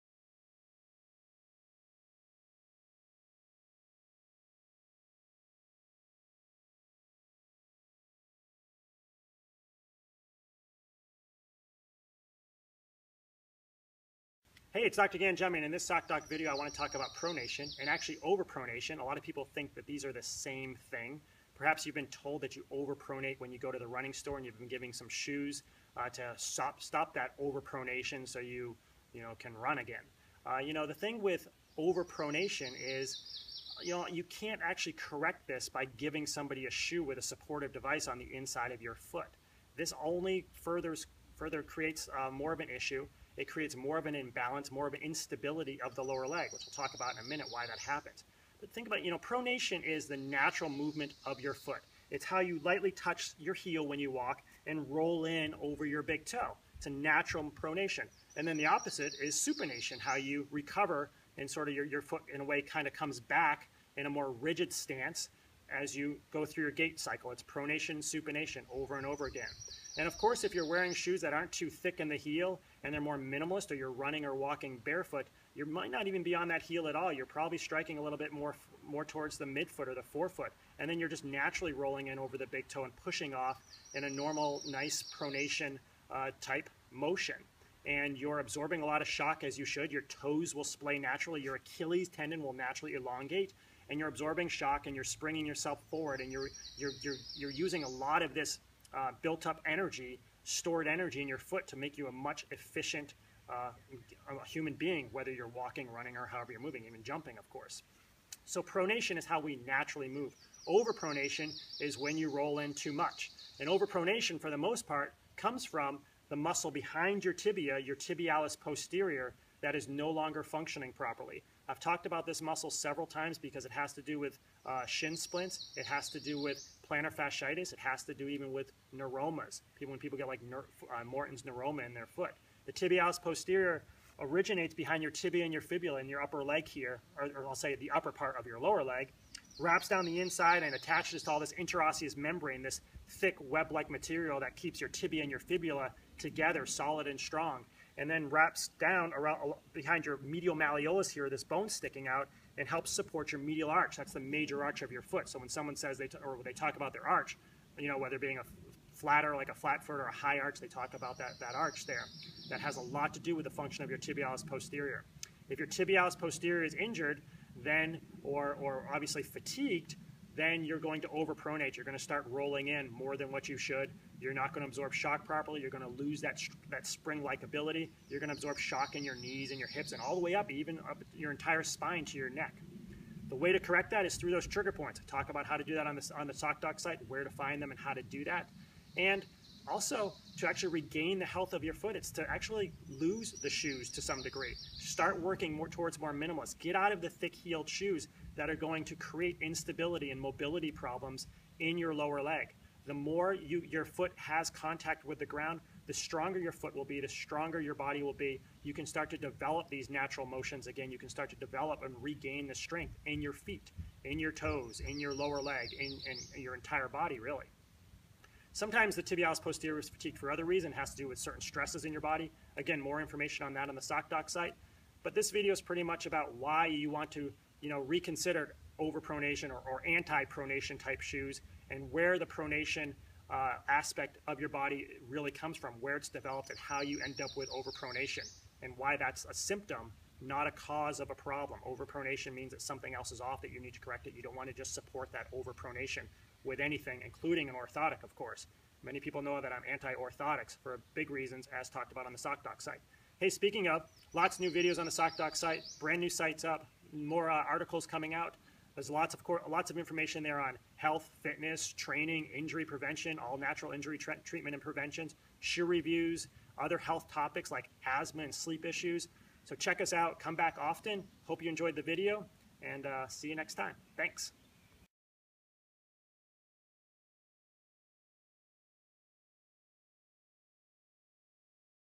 Pronation & Overpronation
Learn what pronation and overpronation mean for runners. Overpronation is a symptom of another problem and is often associated with various injuries. Shoes and orthotics will not truly correct your overpronation.

In this video, Dr. Gangemi explains how overpronation is a symptom, indicating that the tibialis posterior muscle is functioning improperly. An improperly functioning tibialis posterior muscle can relate to many lower-leg issues such as shin splints, plantar fasciitis, Morton's Neuroma, a flat foot or a high foot arch. To help the tibialis posterior muscle to function correctly, he suggests the use of minimalist shoes, barefoot running & walking, and other health changes. Watch this video to learn more about overpronation, tibialis posterior fatigue, and how to improve lower-leg stability and natural mobility.